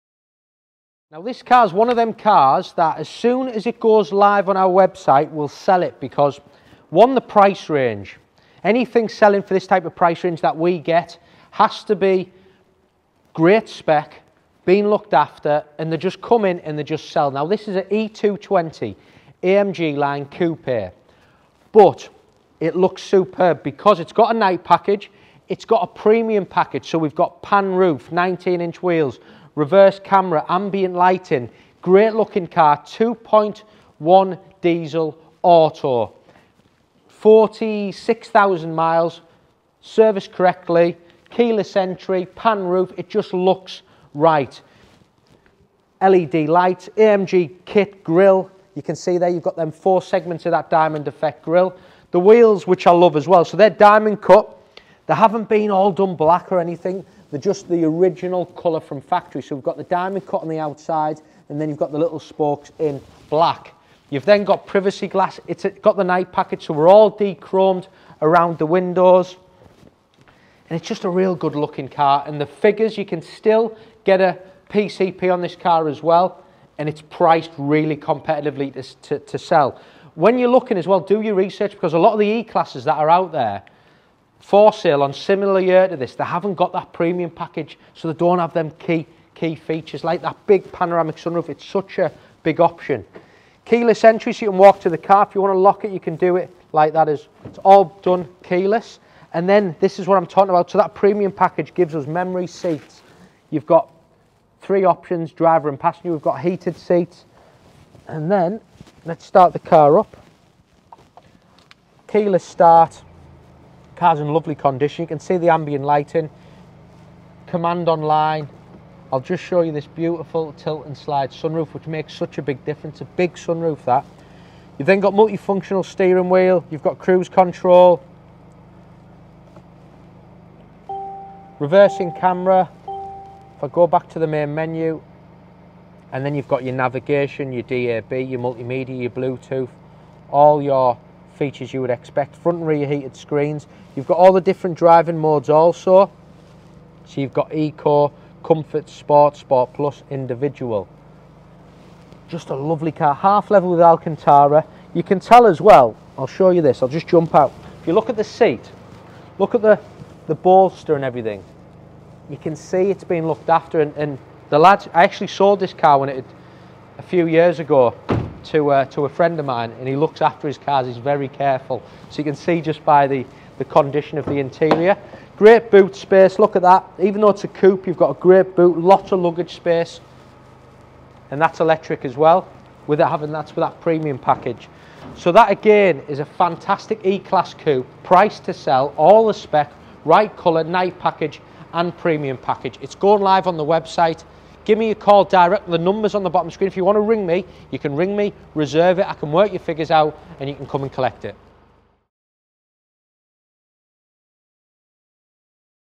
2018 Mercedes E220 2.0D AMG Line Premium Coupe
The perfect combination of style, luxury & efficiency. Introducing our all black E220 Coupe with stealth Night Package Detailing, 19” AMG Alloys & enhanced Premium Package features list including must have Panoramic Glass Sunroof. Be quick with this one..

Please call Liam on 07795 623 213 to enquire about this vehicle 📲